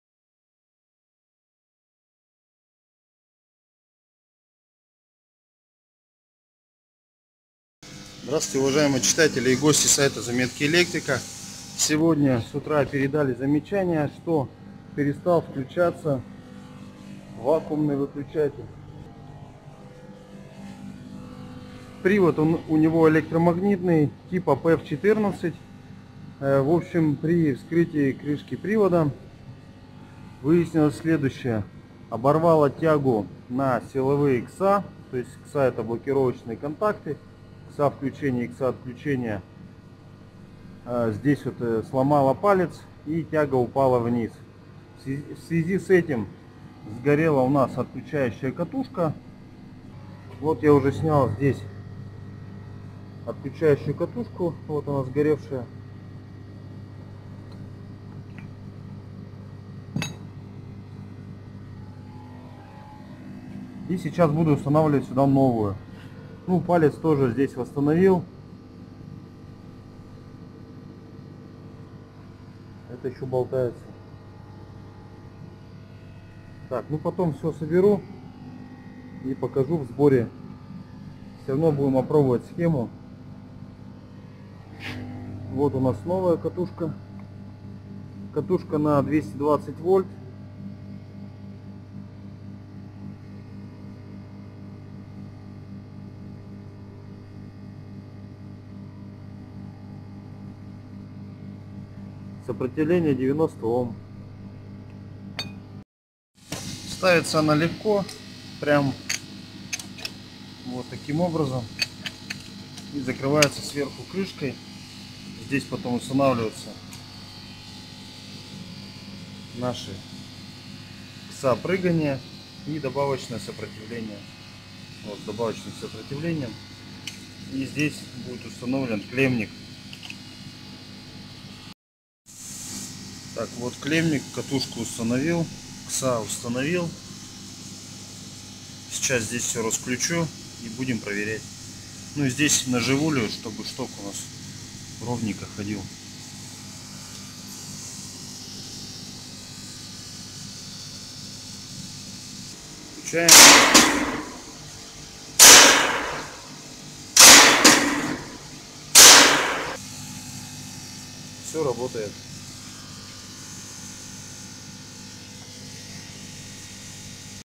Вакуумный выключатель ВБПЭ-10. Привод ПЭВ-14. Сгорела катушка
Пример неисправности электромагнитного привода вакуумного выключателя ВБПЭ-10. Оборвало тягу и сгорела отключающая катушка (ОК). Краткий обзор восстановления работы привода и его опробование.